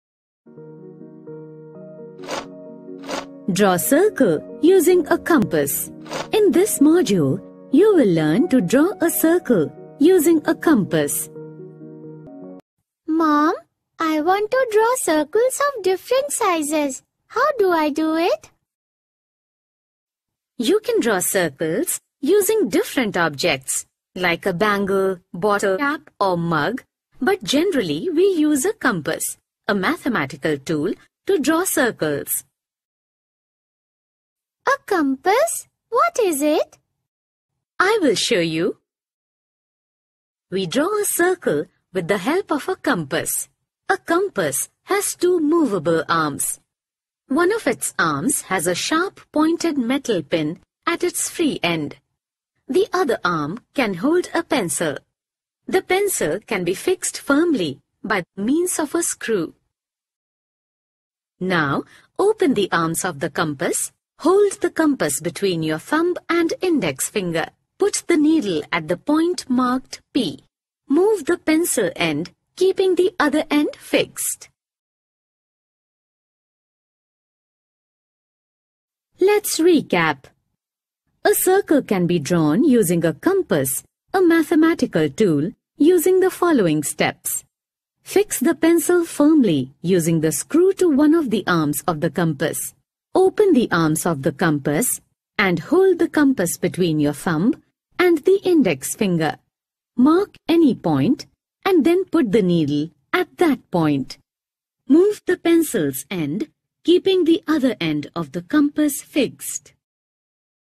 Carts and Wheels | Draw Circle Using a Compass | Math | Class 4
This module explains how to draw a circle using a compass.